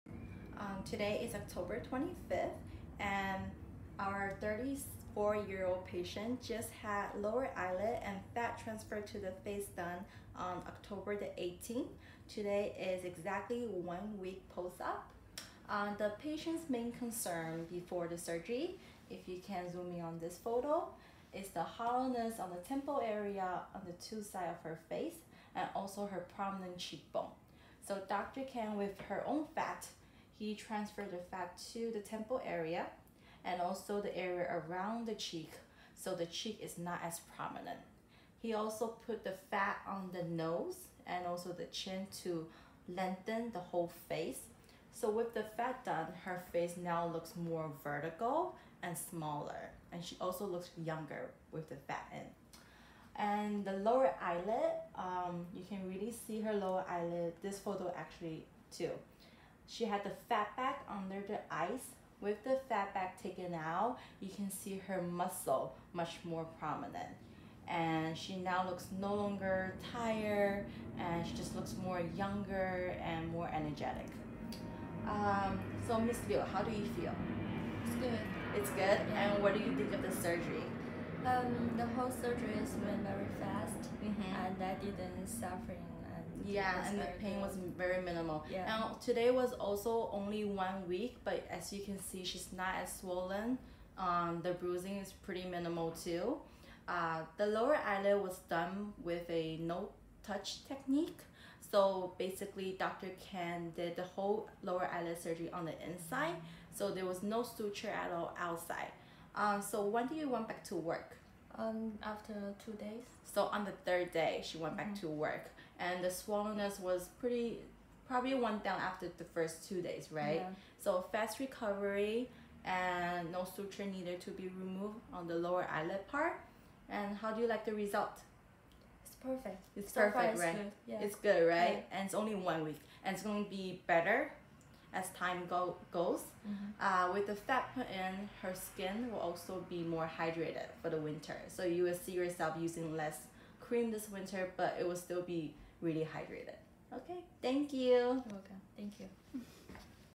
Patient Review: 34 year old. Lower Eyelid Fat Bag Removal and Fat Transfer 1 Week Post Operation
Patient wanted to look more rejuvenated and to contour her face, which lost a lot of volume and became wider as she aged.  After consulting with Dr. Kang, she decided to unlock the journey to youth with removal of her fat bag, which was making her look very tired and exhausted, and fat transfer, which would contour her face and fill in what was lost as a result of aging. After exactly 1 week of the procedure, both done on the same day, she exclaims that the results are “perfect”.